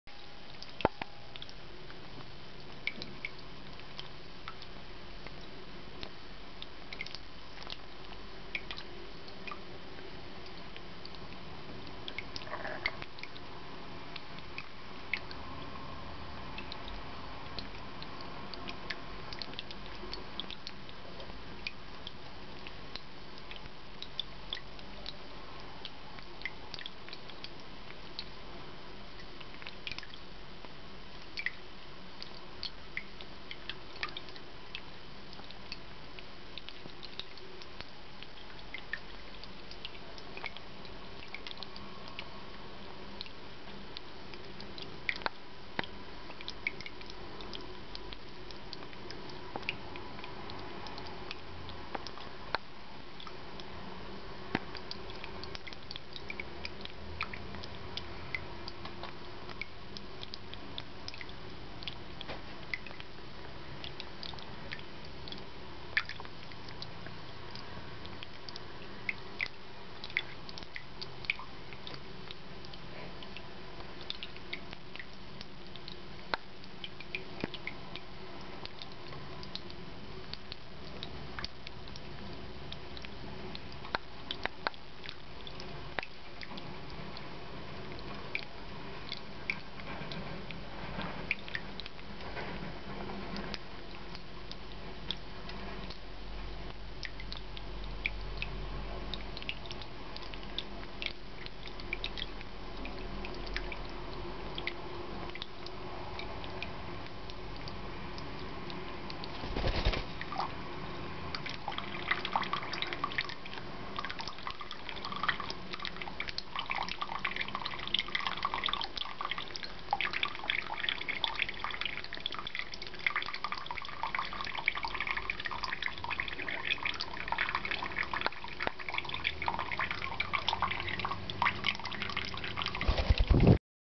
Корелла на настольном фонтанчике
Попугайчик корелла в восторге от фонтанчика:)